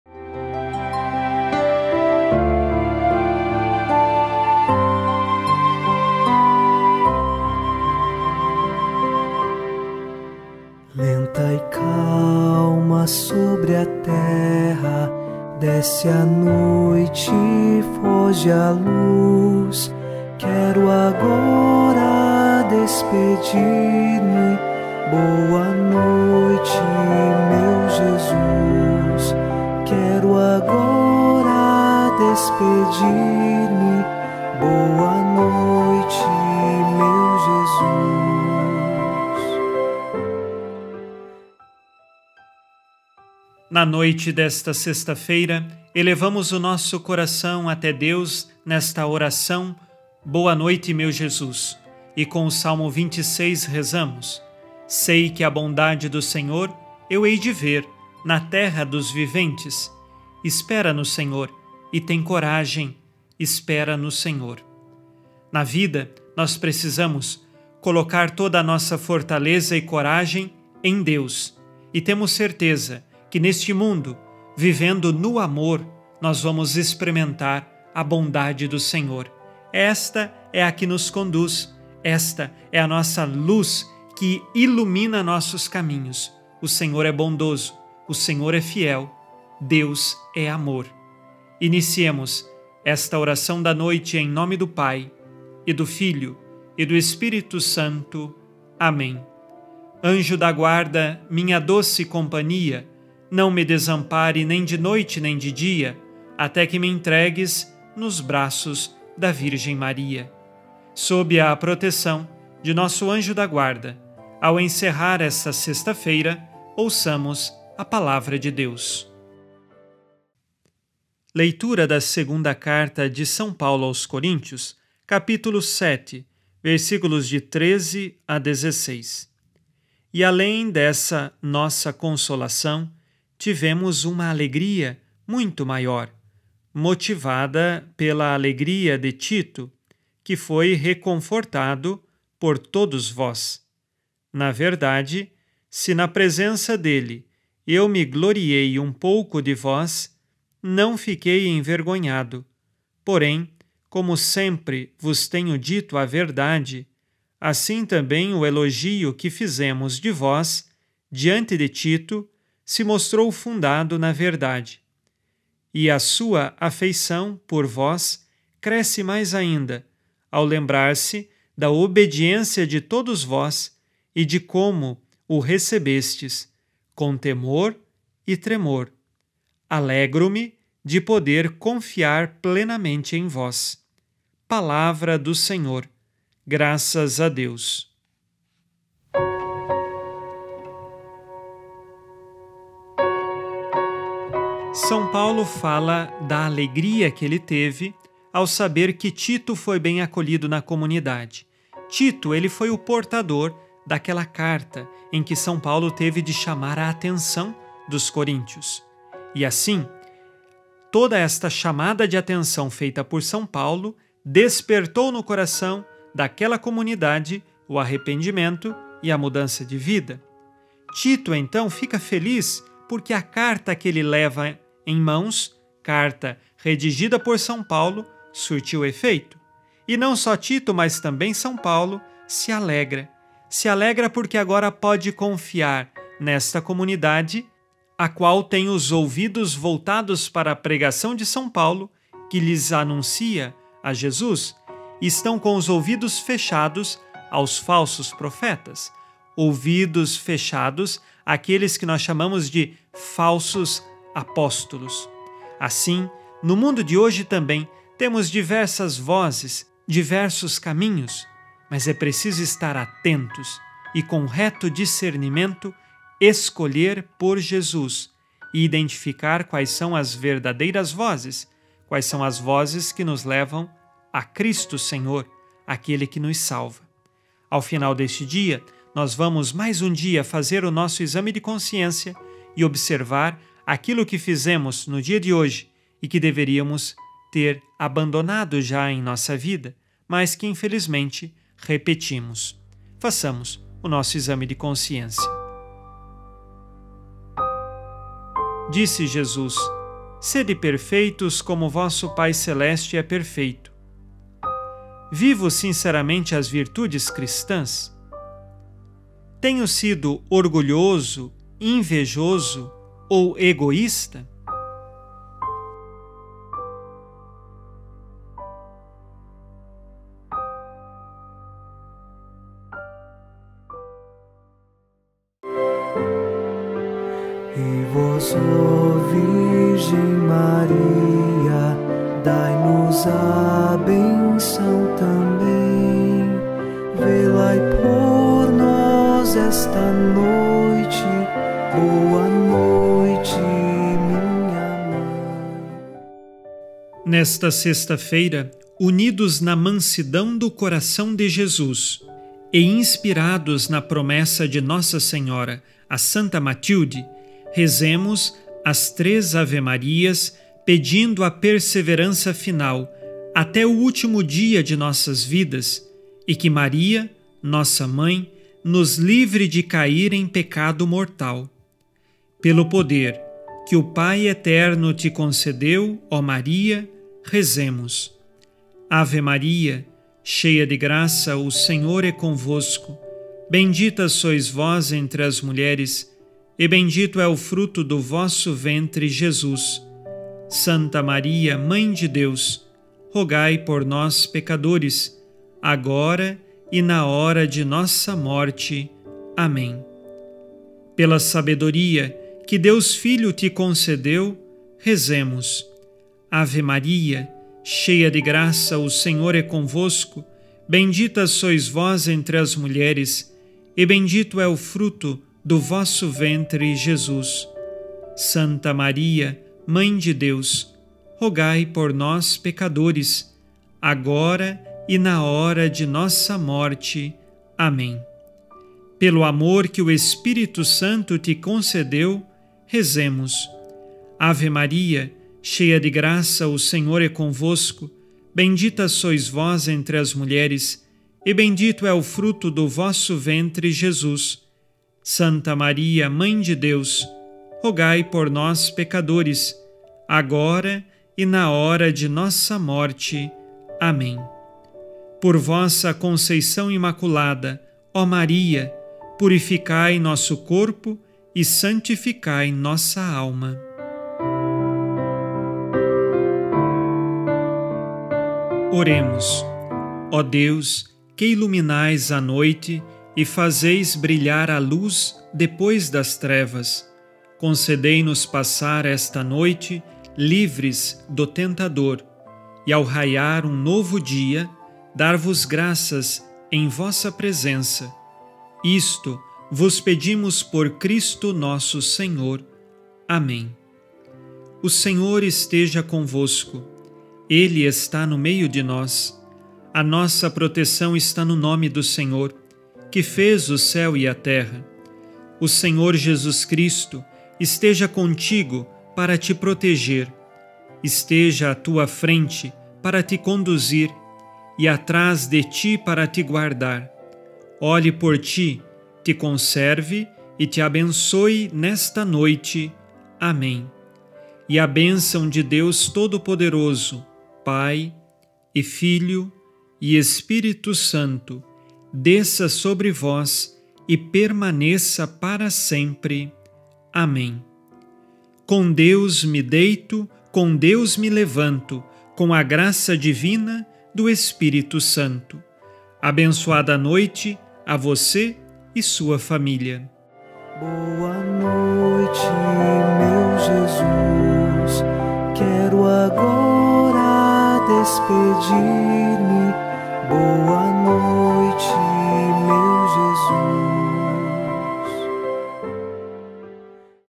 26/04 - Oração da Noite: "Boa Noite, Meu Jesus"
Doação pelo PIX: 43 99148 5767. Colabore na construção da Igreja e Mosteiro Preciosíssimo Sangue.

Todas as noites reze conosco no programa "Boa noite meu Jesus". Oração, meditação da Palavra de Deus, e Benção da noite com Padre Alex Nogueira.